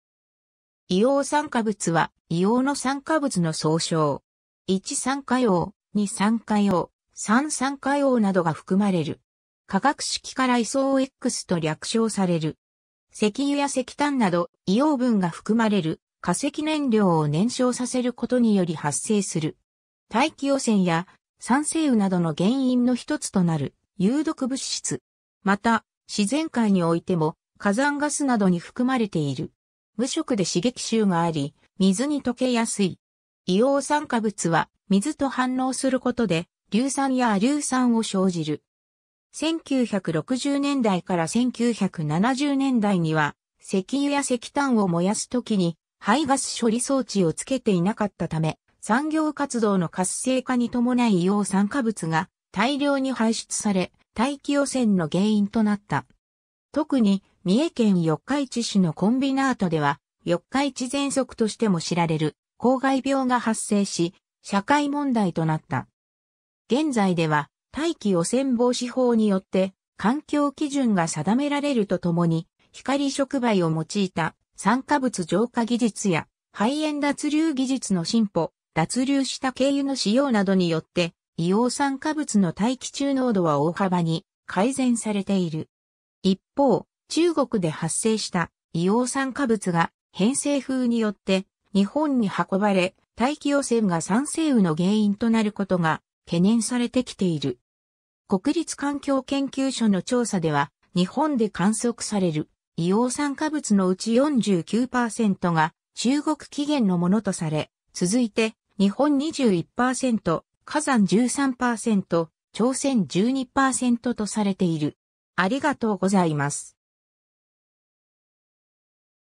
硫黄酸化物
硫黄酸化物（いおうさんかぶつ、英: sulfur oxide）は、硫黄の酸化物の総称。
一酸化硫黄 (SO)、二酸化硫黄（亜硫酸ガス）(SO2)、三酸化硫黄 (SO3) などが含まれる。
化学式から SOx （ソックス）と略称される。
 石油や石炭など硫黄分が含まれる化石燃料を燃焼させることにより発生する。
大気汚染や酸性雨などの原因の一つとなる有毒物質。
また、自然界においても火山ガスなどに含まれている。
無色で刺激臭があり、水にとけやすい。
 硫黄酸化物は水と反応することで、硫酸や亜硫酸を生じる。
 1960年代から1970年代には、石油や石炭を燃やすときに排ガス処理装置をつけていなかったため、産業活動の活性化に伴い硫黄酸化物が大量に排出され、大気汚染の原因となった。
特に三重県四日市市のコンビナートでは、四日市ぜんそくとしても知られる公害病が発生し、社会問題となった。
現在では、大気汚染防止法によって環境基準が定められるとともに、光触媒を用いた酸化物浄化技術や排煙脱硫技術の進歩、脱硫した軽油の使用などによって、硫黄酸化物の大気中濃度は大幅に改善されている。
 一方、中国で発生した硫黄酸化物が偏西風によって日本に運ばれ、大気汚染が酸性雨の原因となることが懸念されてきている。
国立環境研究所の調査では、日本で観測される硫黄酸化物のうち 49% が中国起源のものとされ、続いて日本 21%、火山 13%、朝鮮 12% とされている。